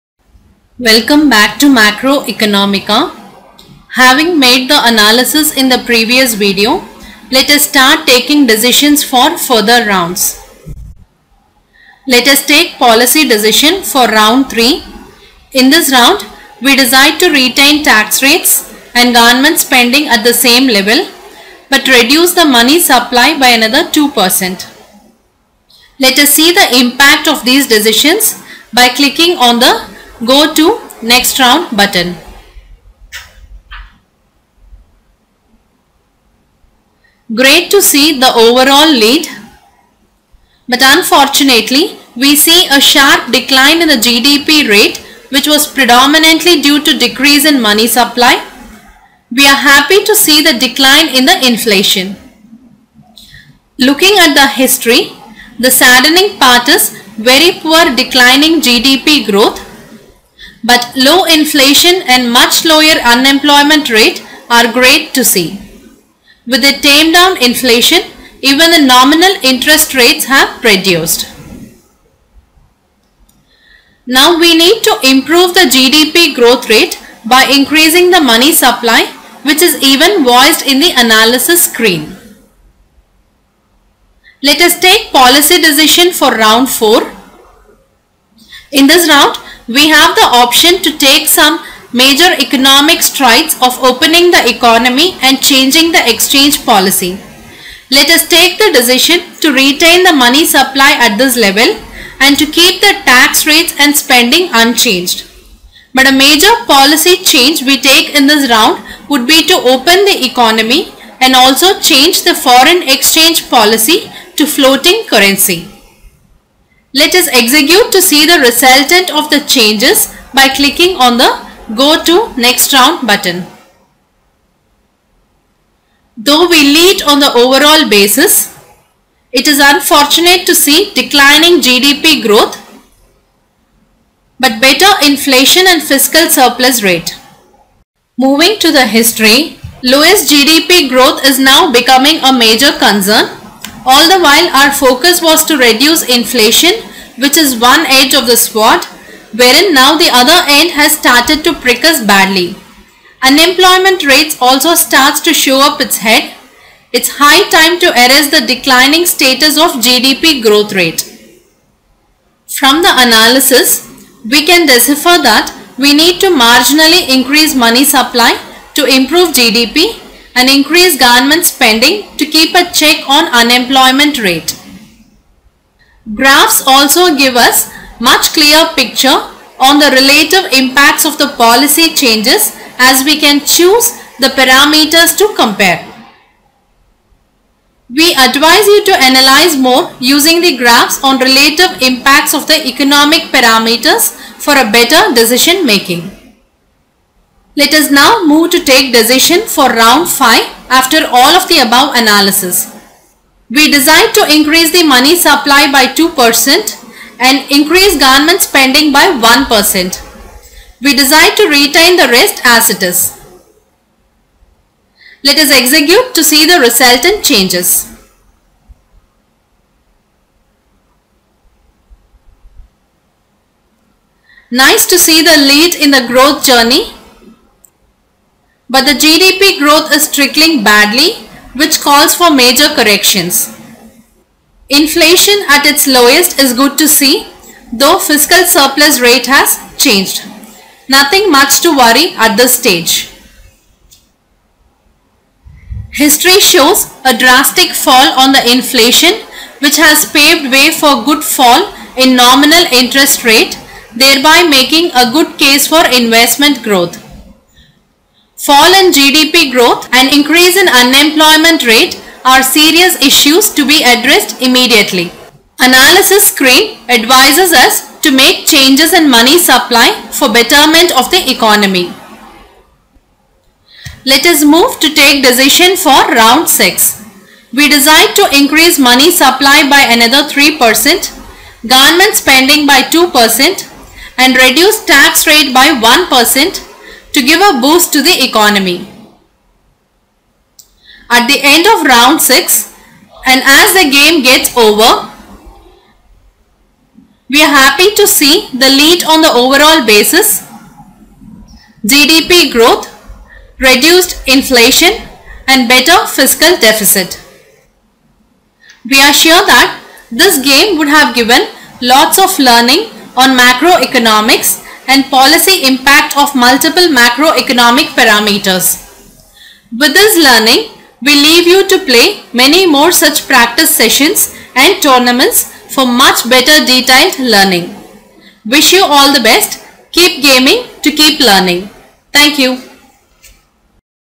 MacroEconomica - Taking Decisions2
Teaching Video for MacroEconomica - Macro Economics Simulation from LearnBiz Simulations